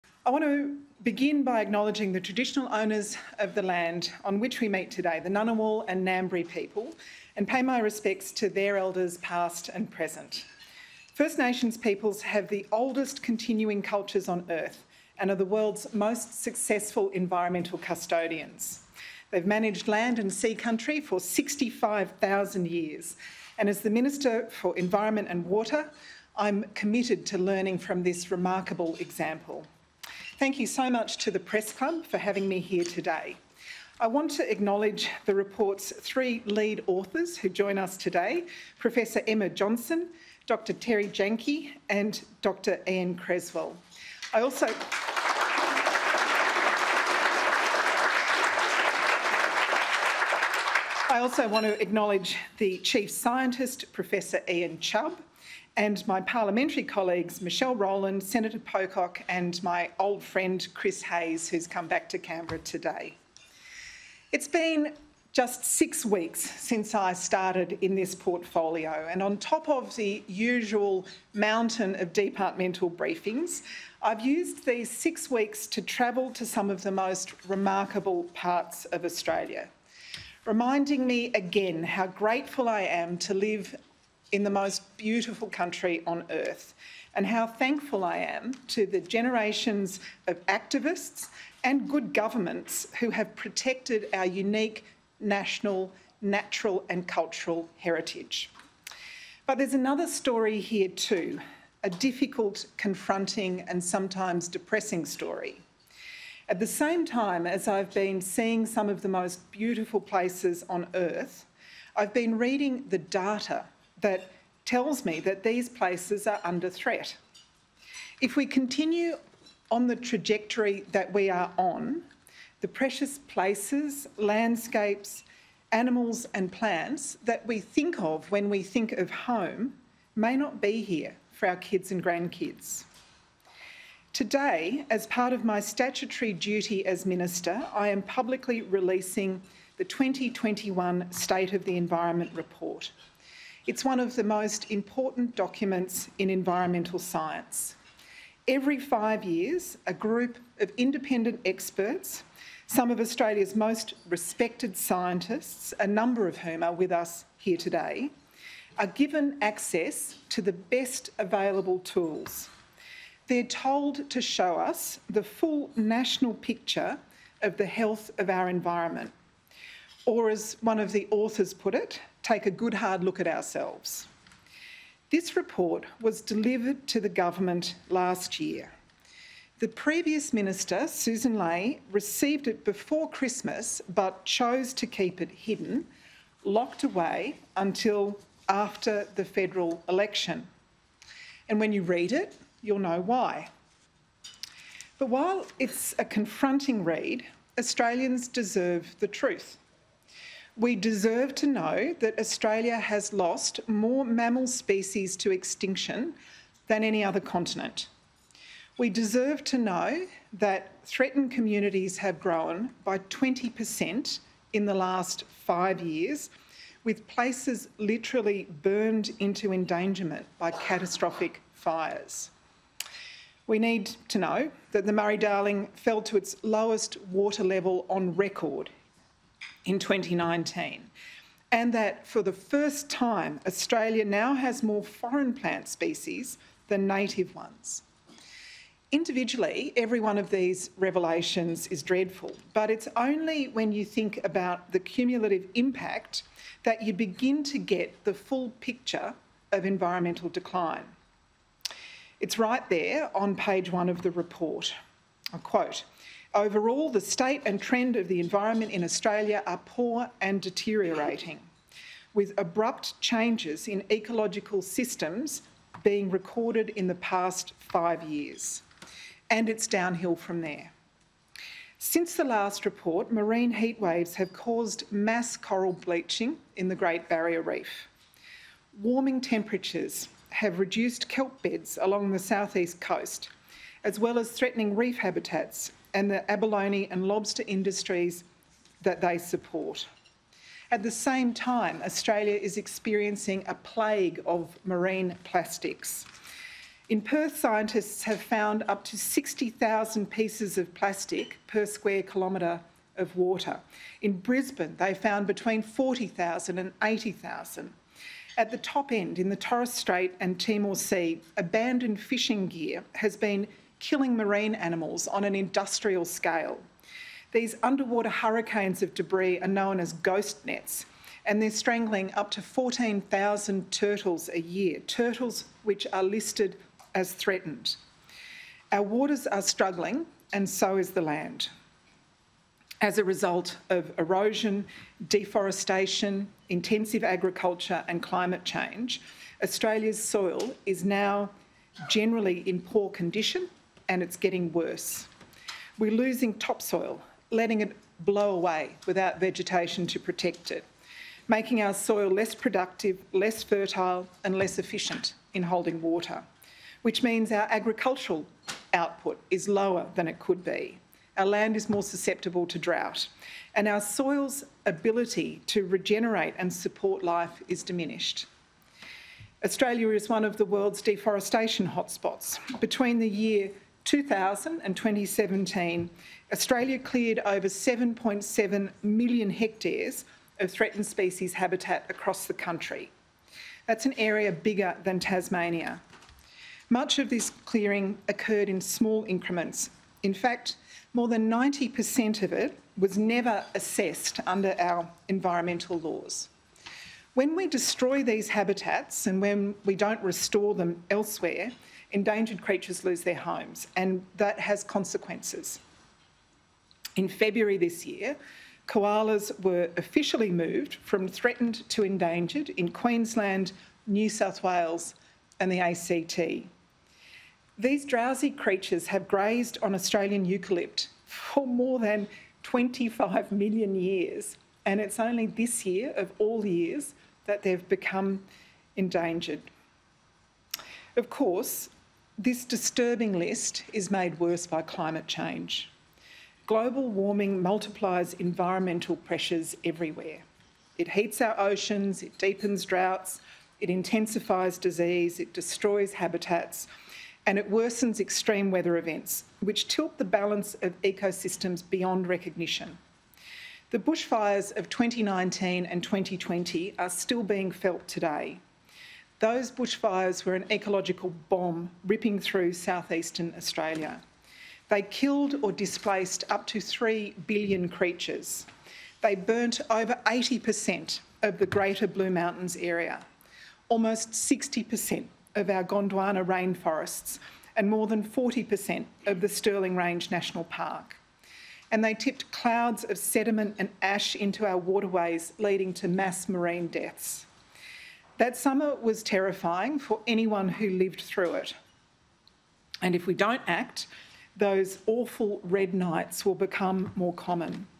'Global warming multiplies environmental pressures everywhere': Tanya Plibersek
Environment Minister Tanya Plibersek says global warming “multiplies environmental pressures” everywhere.

“It heats our oceans, it deepens droughts, it intensifies disease, it destroys habitats, and it worsens extreme weather events which tilt the balance of ecosystems beyond recognition,” Ms Plibersek said.

“The bushfires of 2019 and 2020 are still being felt today.

“Those bushfires were an ecological bomb, ripping through south-eastern Australia.”